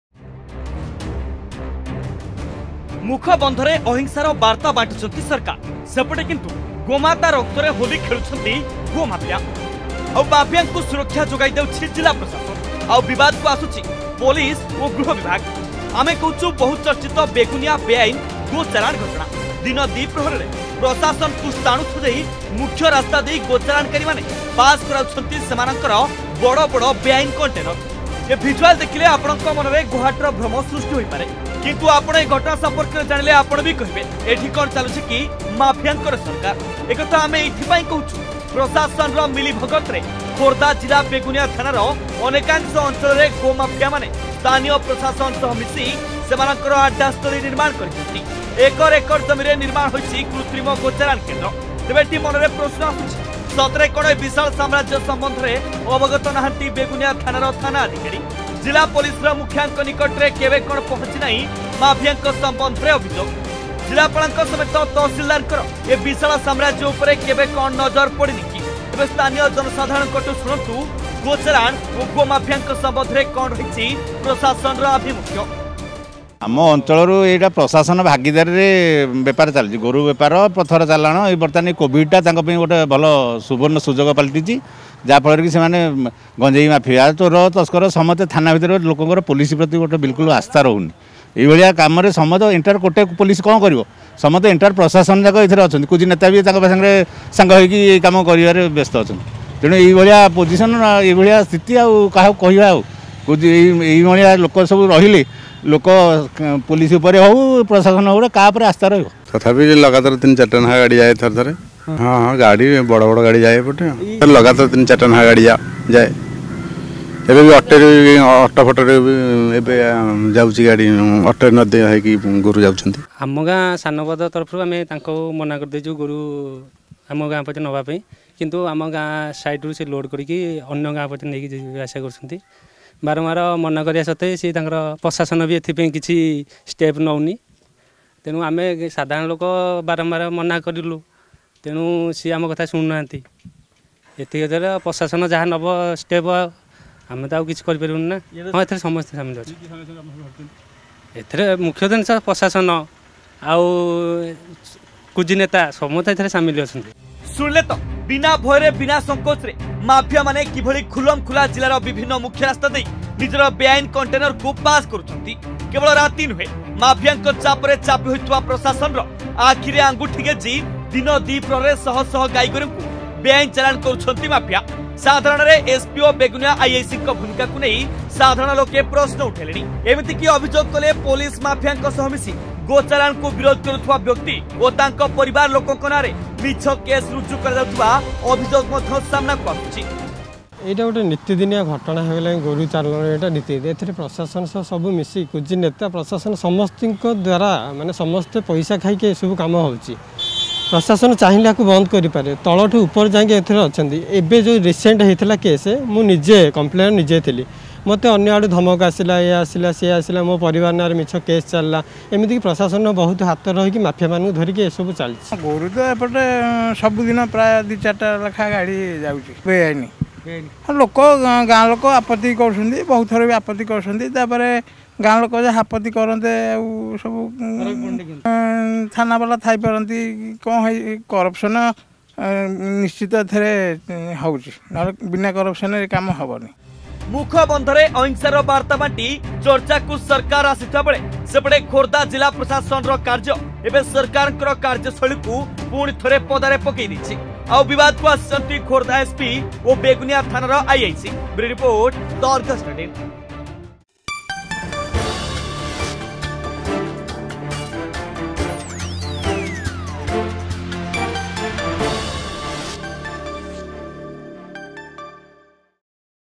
ଗୋମାତା ରକ୍ତରେ ହୋରି ଖେଳୁଛନ୍ତି ଗୋ-ମାଫିଆ ....
ଦିନଦ୍ୱିପହରରେ ଗୋରୁ ଚାଲାଣ, ପ୍ରଶାସନର ଛତ୍ରଛାୟା ତଳେ ଚାଲିଛି ବେଆଇନ ଗୋ ଚାଲାଣ. ଅତି ବିପଦସଙ୍କୁଳ ଅବସ୍ଥାରେ କଣ୍ଟେନର ମାଧ୍ୟମରେ ପ୍ରତିଦିନ ଚାଲାଣ ହେଉଛନ୍ତି ଶହଶହ ଗୋରୁ, ଆଉ ସୁରକ୍ଷା ଦେଇ ଚର୍ଚ୍ଚାକୁ ଆସିଛି ଖୋର୍ଦ୍ଦା ପ୍ରଶାସନ.